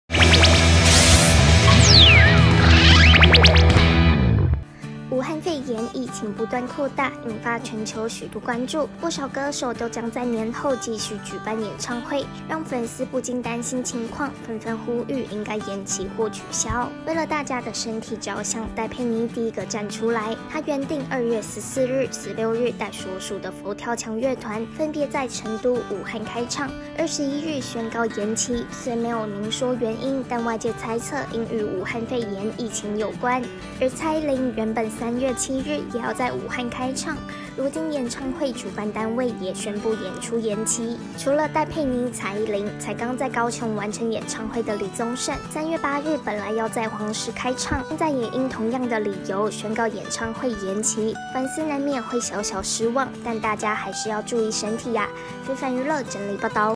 武漢肺炎蔓延 蔡依林戴佩妮演唱會延期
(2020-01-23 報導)

日前，因武漢新型冠狀病毒肺炎疫情的發生，不少人的生活都受到了影響，娛樂圈中不少明星也是如此，1月22日，蔡依林演唱會武漢站就宣佈因此事延期。